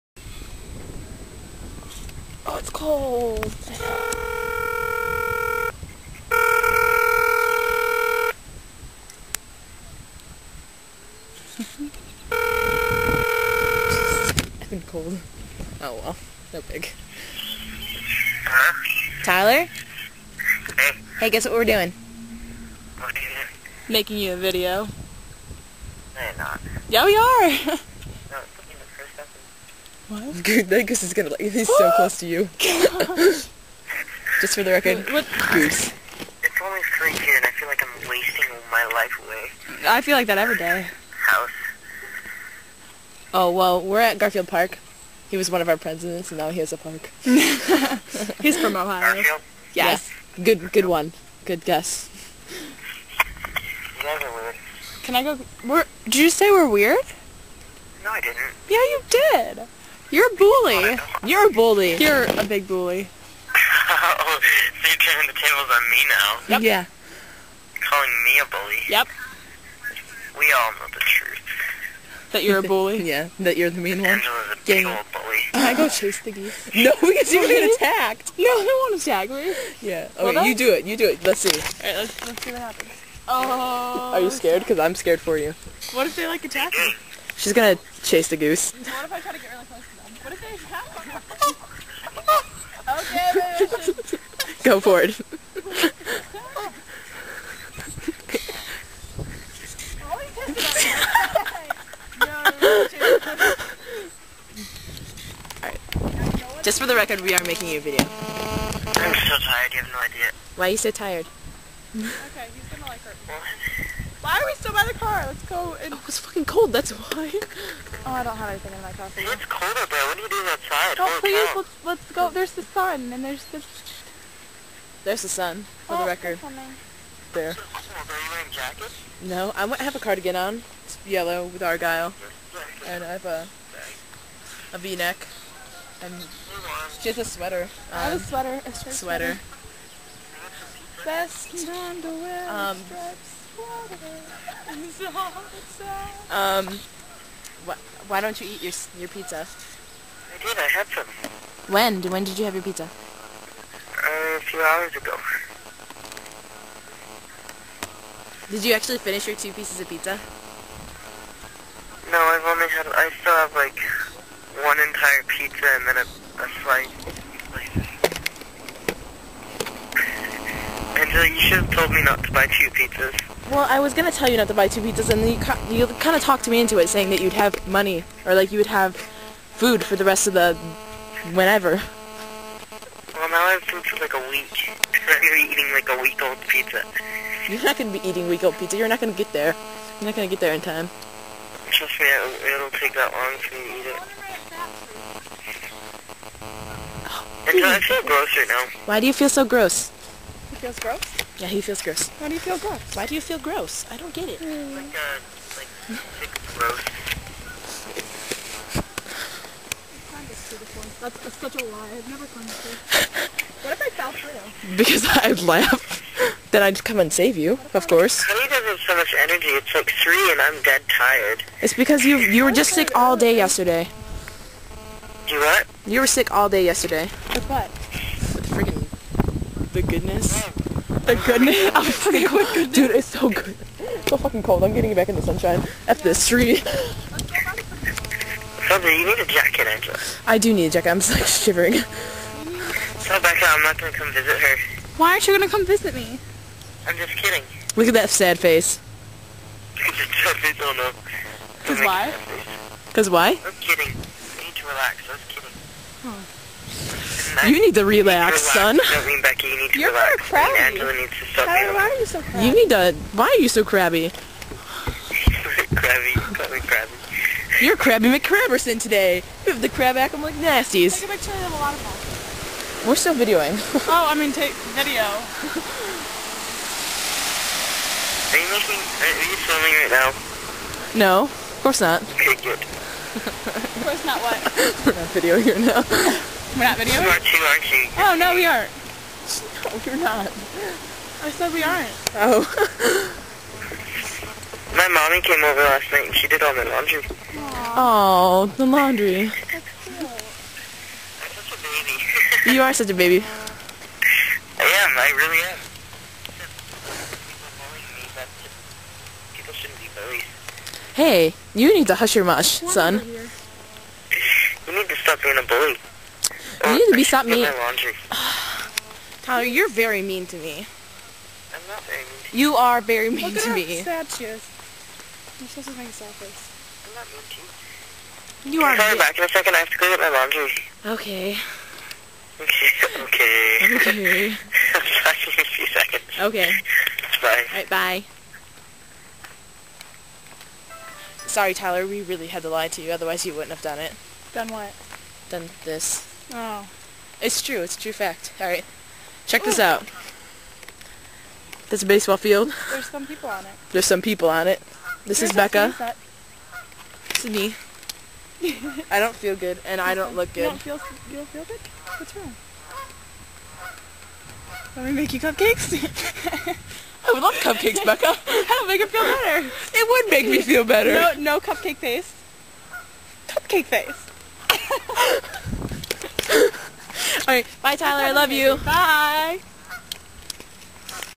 Phone call with Tyler
Becca and Angela trick Tyler into having a phone conversation on film.